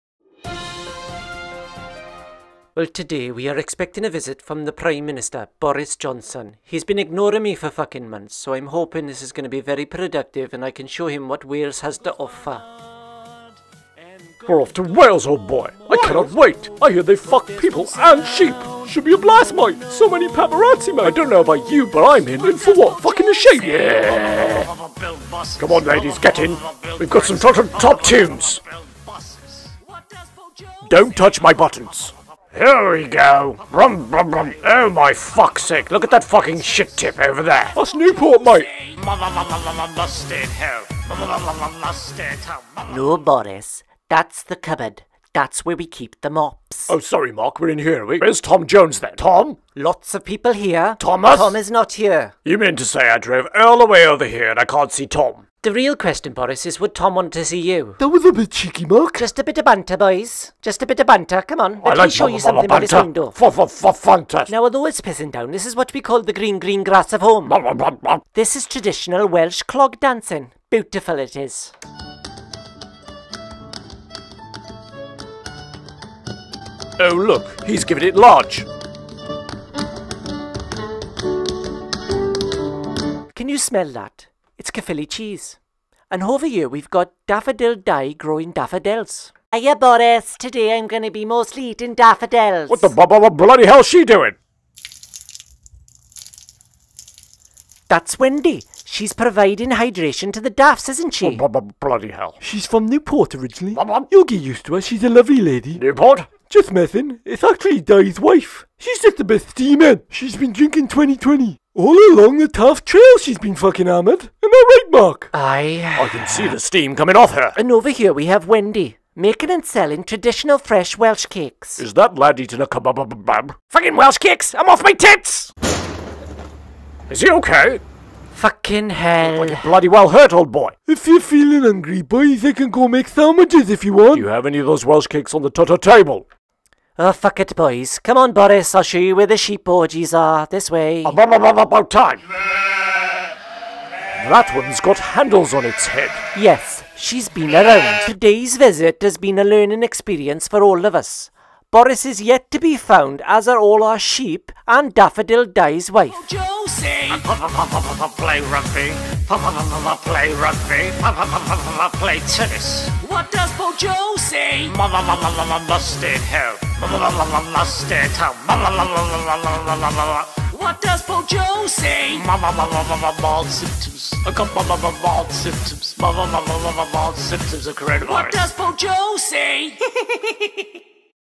Bojo visits Mark in Cardiff. -Parody
Boris visits Mark in Cardiff. Mark shows Boris the beautiful Welsh culture with the odd negative interruption. Joke. I am from Wales..

Socials